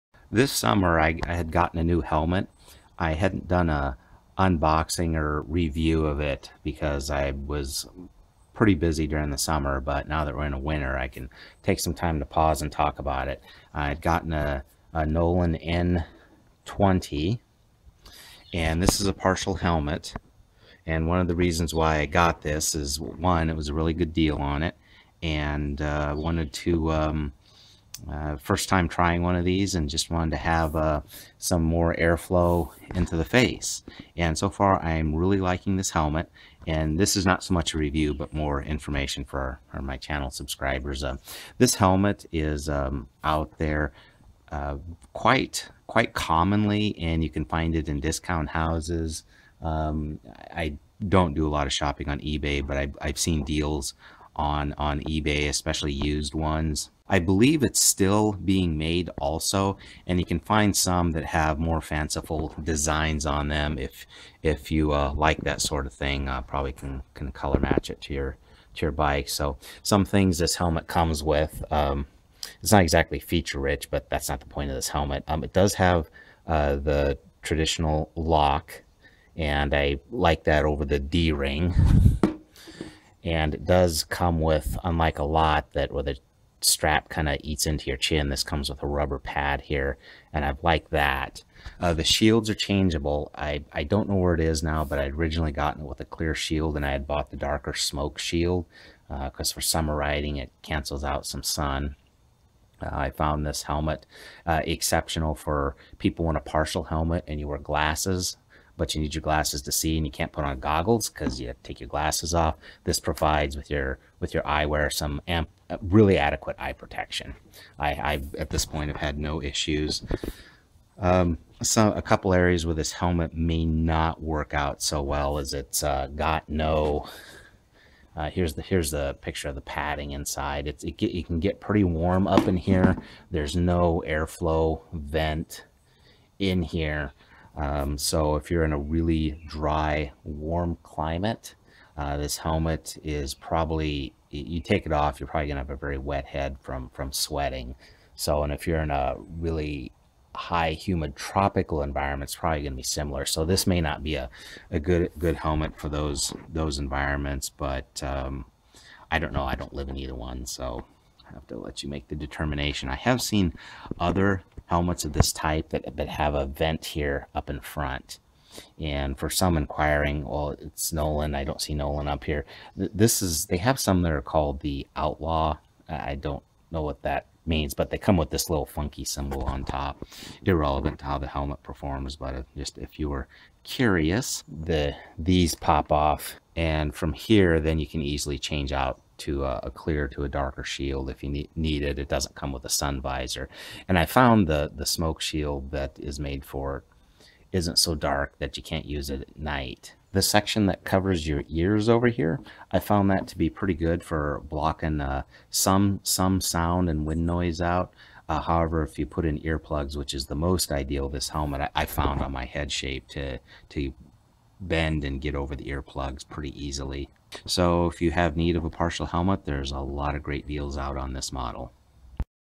Nolan N20 Motorcycle Helmet
Introducing a new motorcycle helmet I purchased, a Nolan N20.